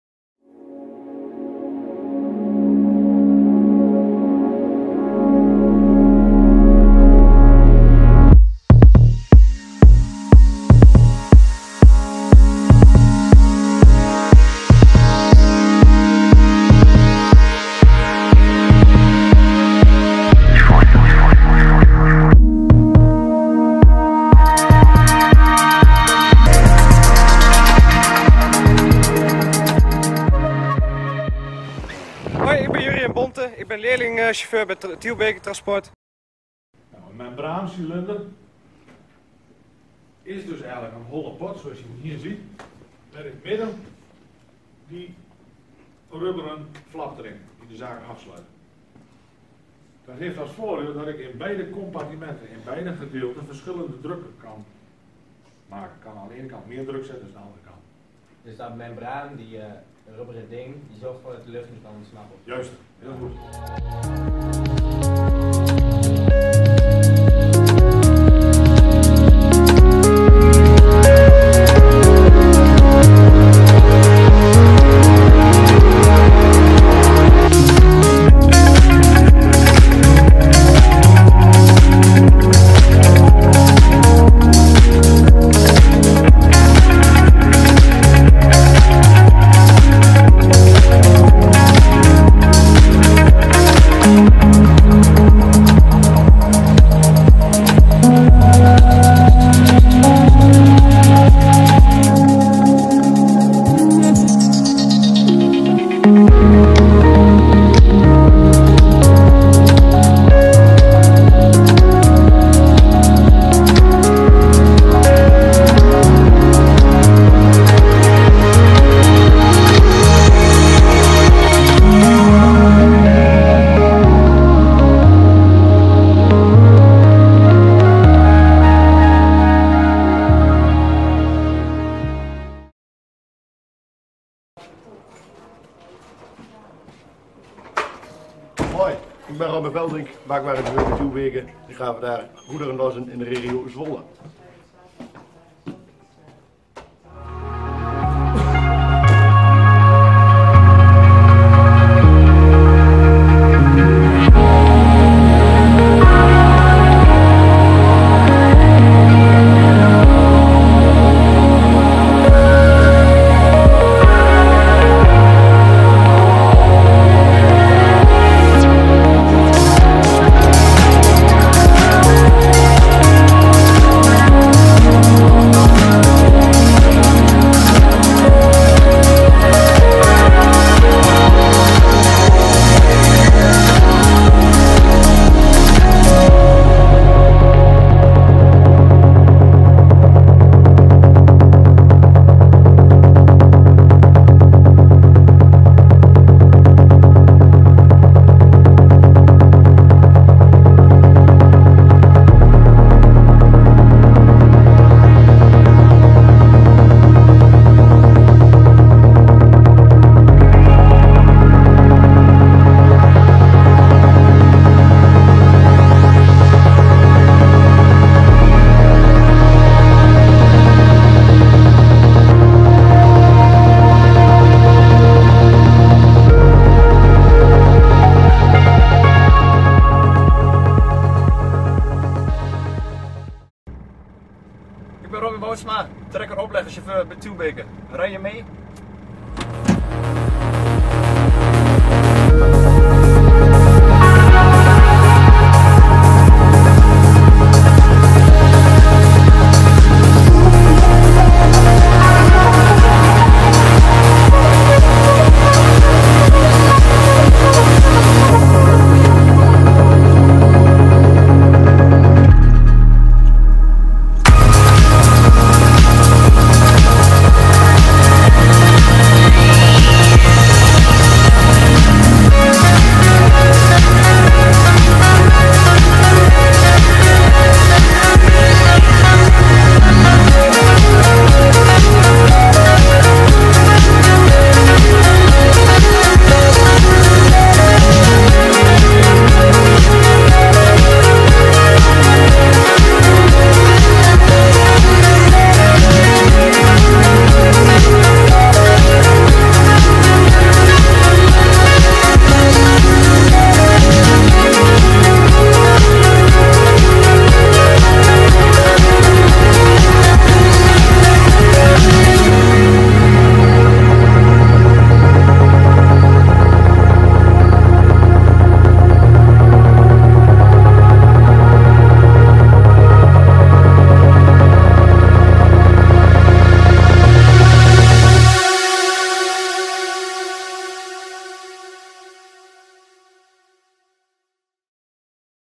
Een dag in het leven van... een Tielbekechauffeur!
Tijdens de Inspiratiedag van Tielbeke hebben vele geïnteresseerden onze 360-graden-video bekeken door een van Virtual Reality-brillen. De video neemt de kijker me naar een dag in het leven van een Tielbekechauffeur. Van leerling tot chauffeur op een trekker en oplegger.